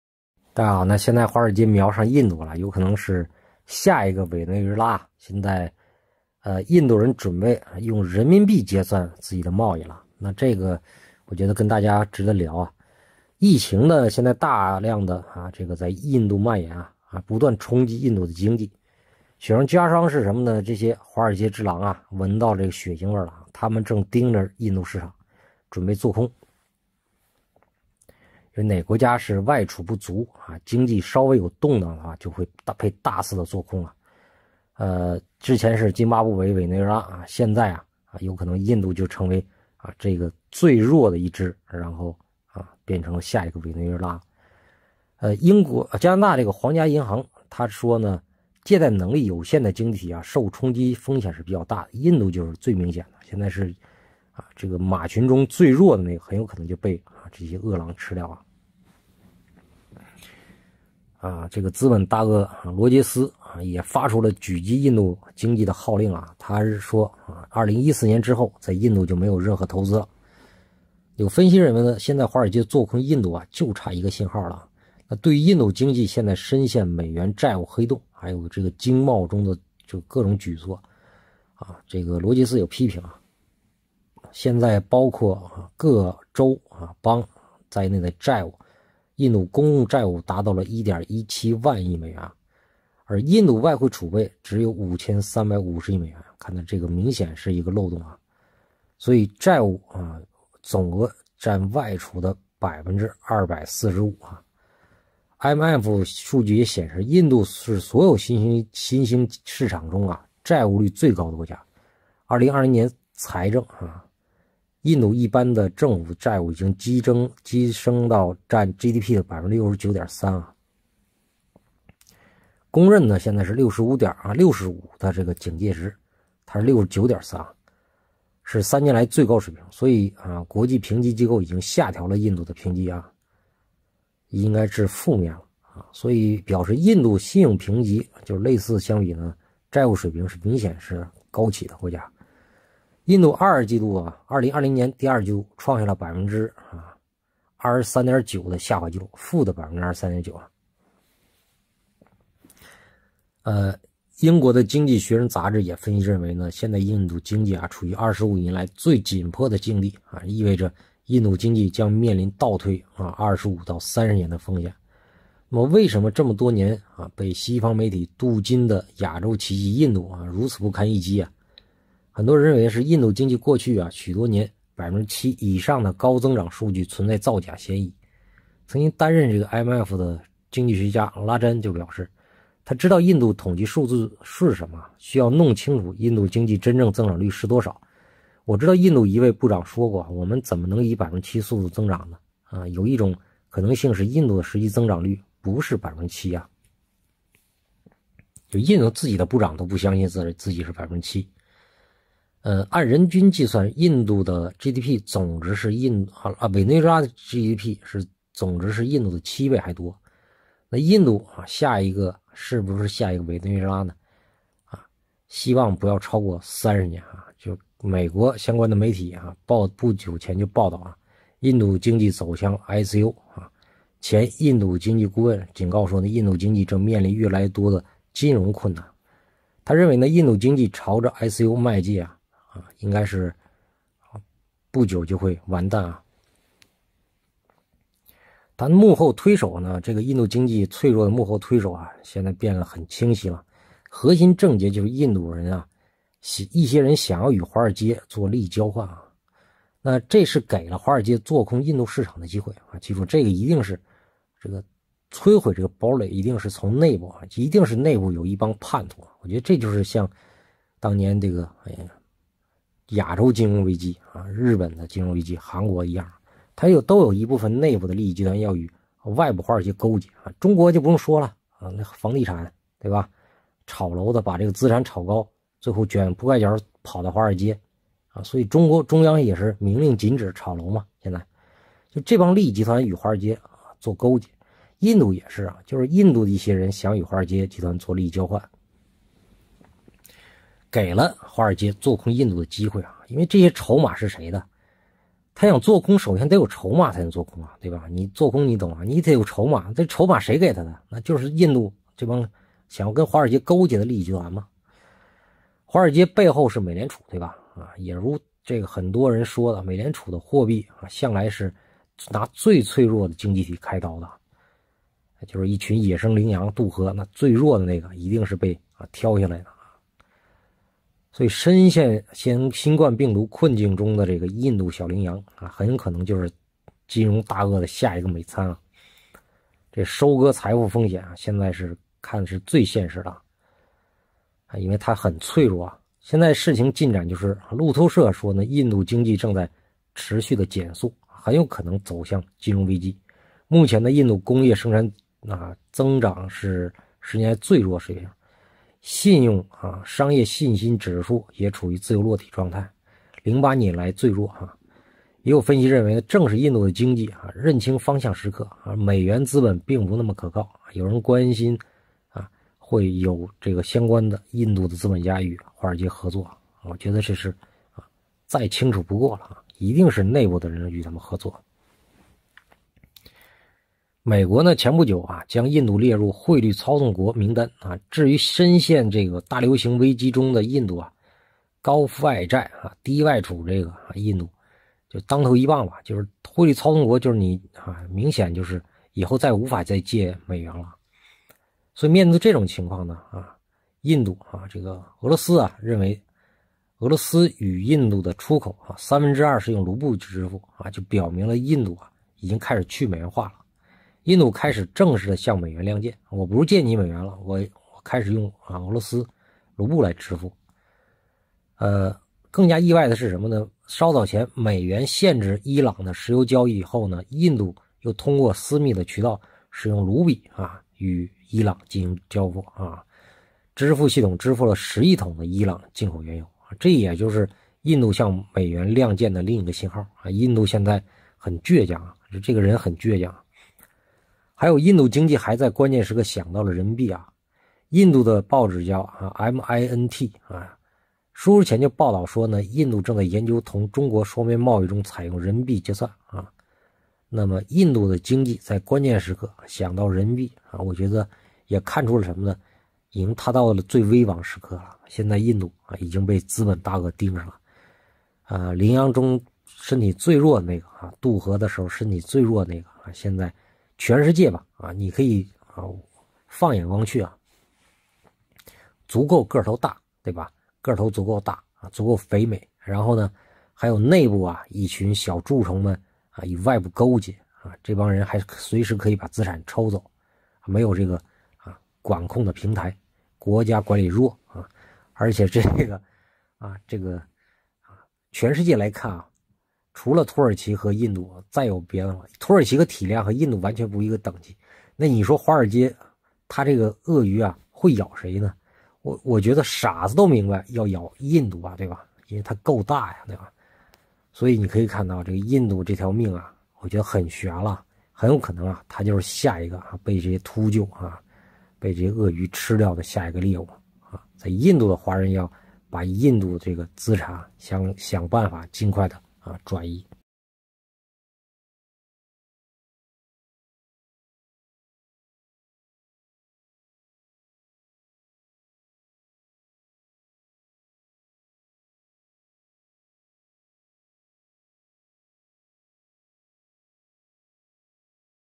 美国华尔街大鳄瞄上 印度，准备#里应外合做空！印度突然决定使用
美国华尔街大鳄瞄上印度，准备里应外合做空！印度突然决定使用人民币结算